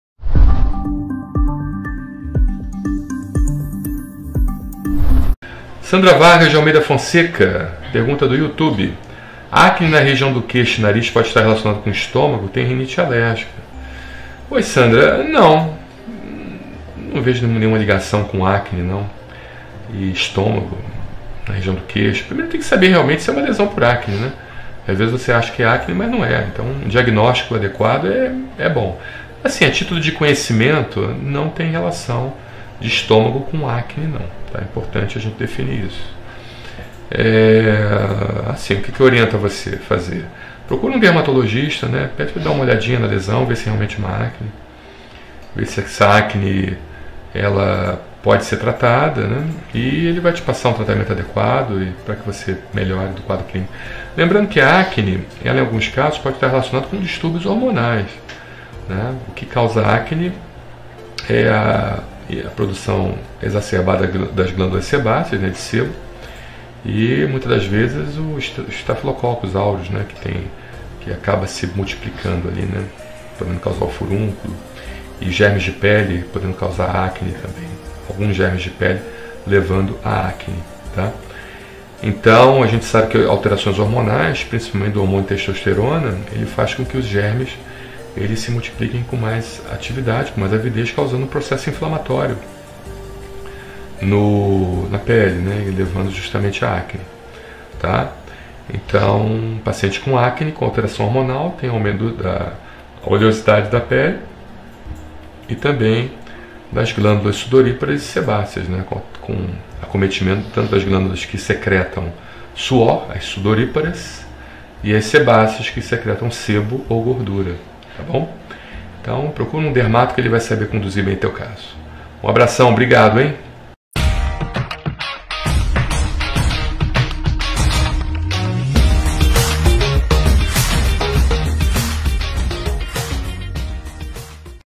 Acne no queixo e nariz, pode estar relacionada com estômago? | Tenho rinite alérgica
Sandra Vargas De Almeida Fonseca - Acne na região queixo e nariz, pode estar relacionada com estômago? Tenho rinite alérgica.

Rinite Alergica:

A rinite alérgica ocorre sazonalmente ou durante o ano todo. O diagnóstico envolve análise do histórico do paciente, exame das passagens nasais e, às vezes, teste cutâneo.
Os sintomas incluem espirros, coriza e olhos vermelhos, lacrimejantes e que coçam.
Medicamentos anti-histamínicos podem reduzir os sintomas.
Consulte um médico para receber orientação
Fontes: Hospital Israelita A. Einstein e outros.